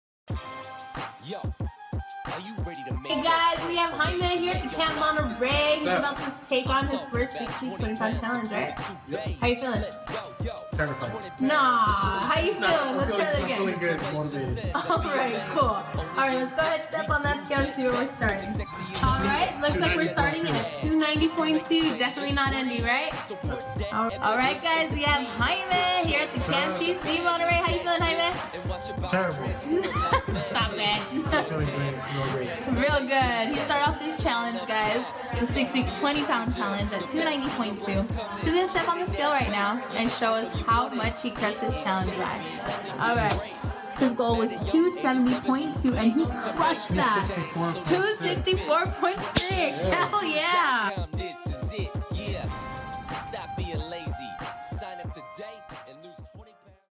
Monterey Weight Loss Fitness 6 Week Challenge Results - Jaime H.
We will now check the result of Jaime's 6 week weight loss challenge!